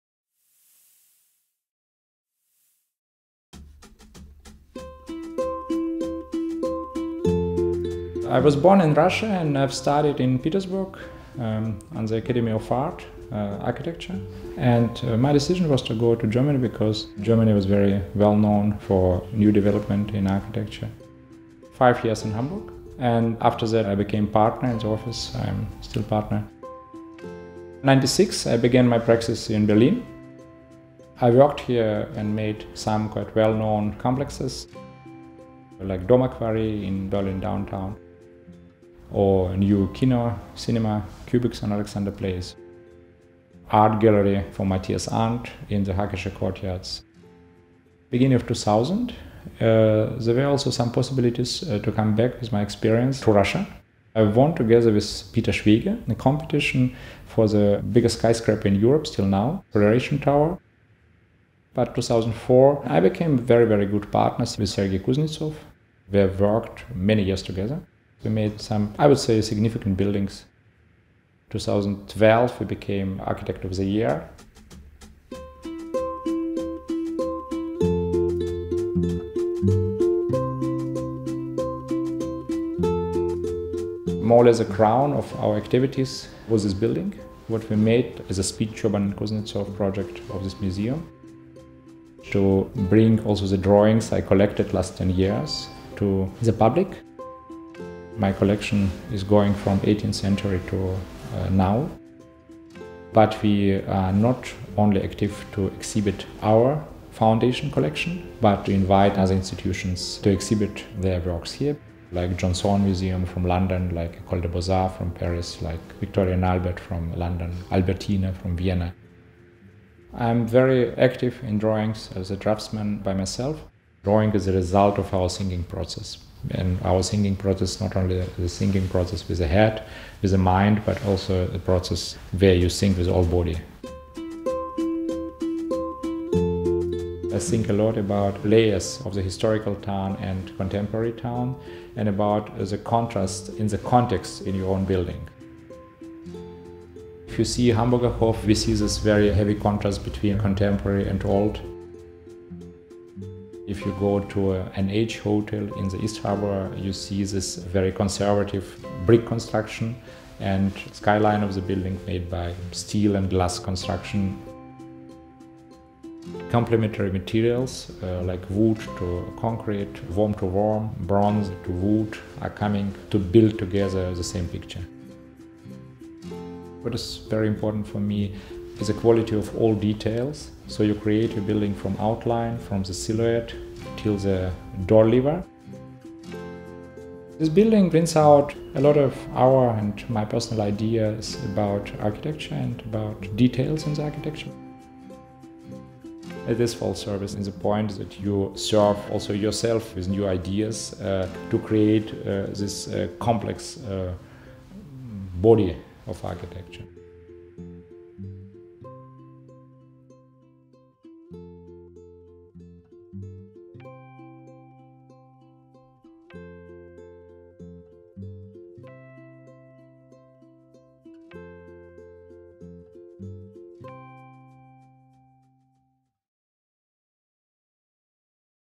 Interview with Sergei Tchoban
Sergei Tchoban, designer of the 2012 Russian Pavilion and Museum of Architectural Drawings in Berlin discusses his work and approach to architecture in this film interview produced by BerlinArtLink and Esenija Bannan.

Interviewee: Sergei Tchoban
Interviewer: Alison Hugill
Editors: Peter Cairns
Produced by: BerlinArtLink & Esenija Bannan